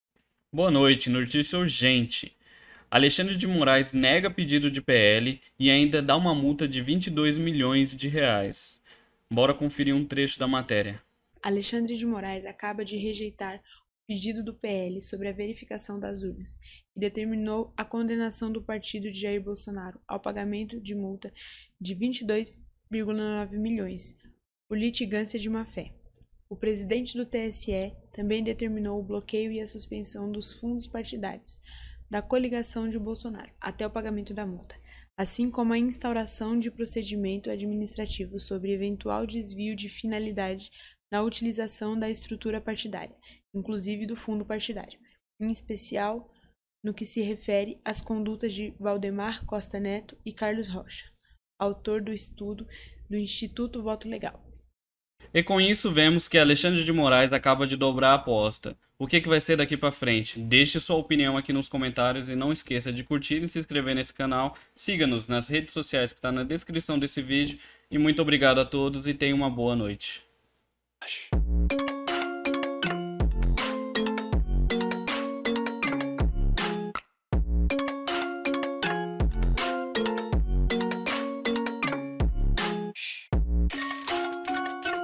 URGENTE: ALEXANDRE DE MORAIS NEGA PEDIDO DO PL, E MULTA EM R$22 MI
Bem Vindo ao Canal Do Jhony